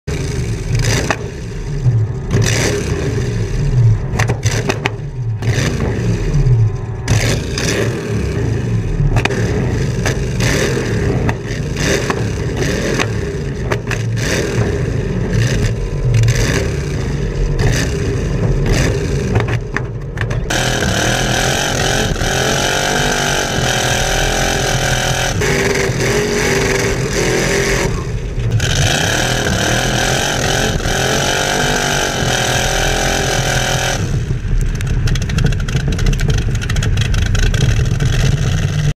33 Chevy Burnouts and flames
1933 Chevrolet Sedan Rat Rod flamethrowers and burnouts